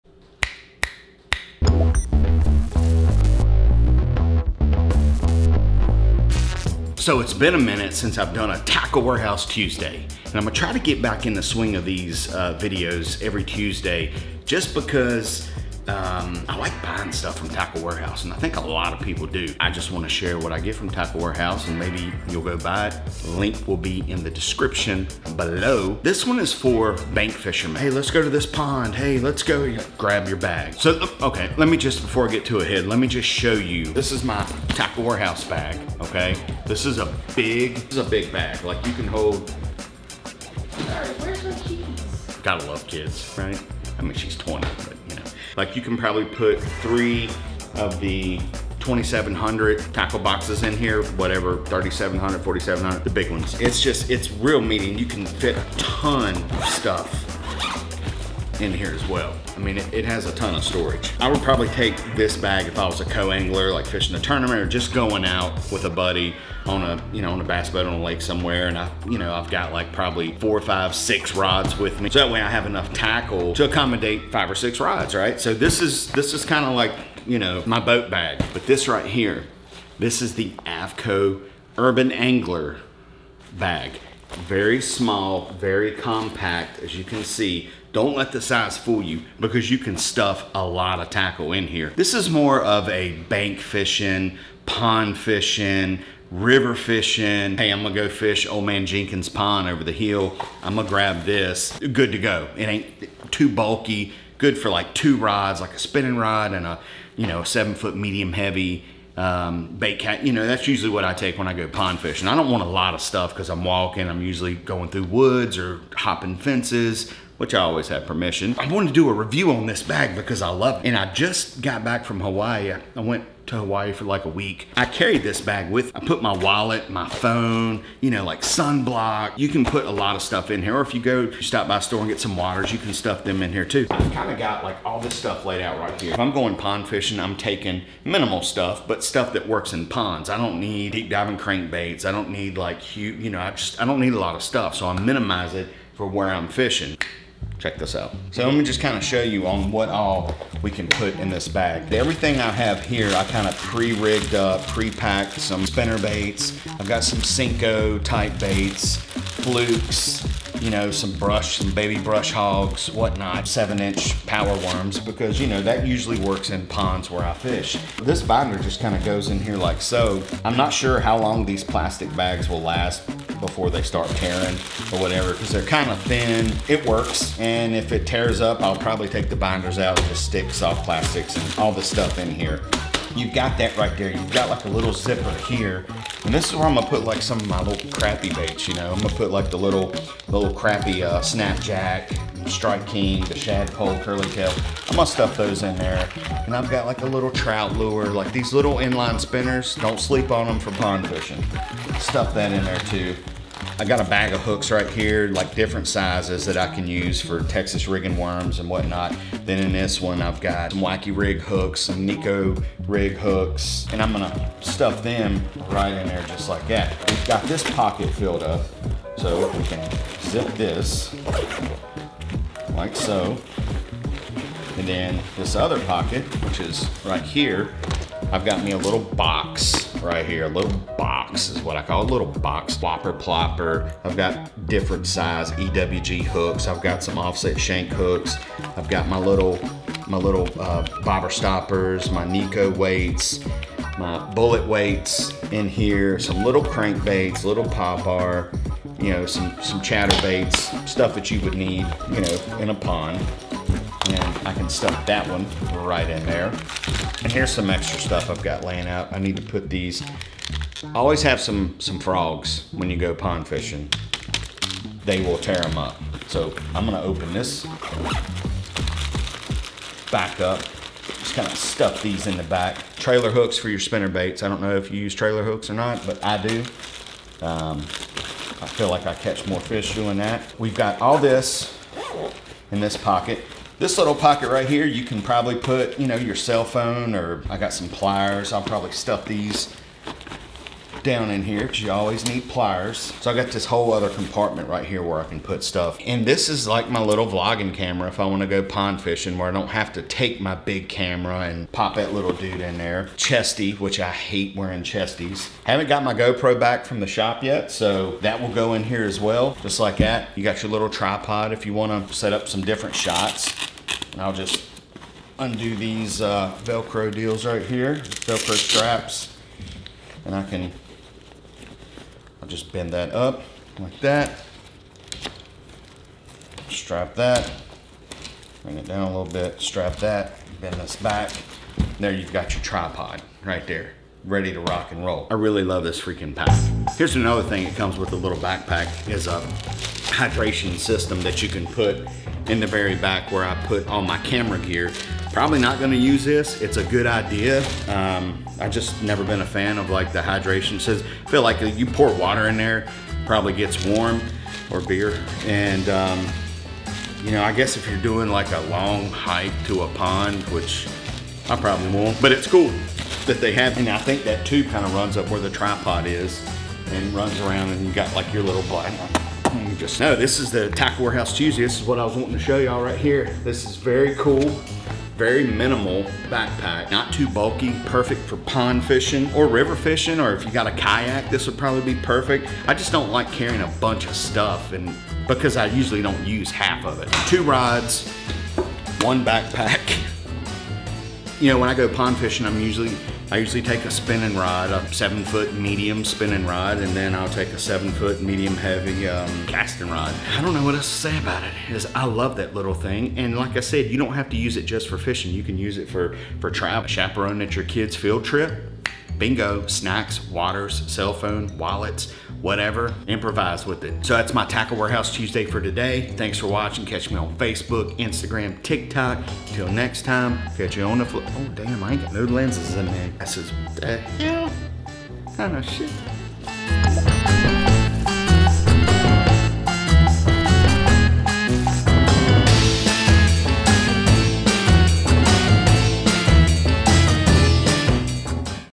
Tackle Warehouse Tuesday: AFTCO Urban Angler Backpack
The Aftco Urban Angler Backpack is a downsized pack with a functional design and plenty of storage for a day of fishing. Perfect for bank anglers, the Aftco Urban Backpack is made from 420D polyester, which is both extremely durable and lightweight. @AFTCO1958

with code DUDE15
with code DLF2022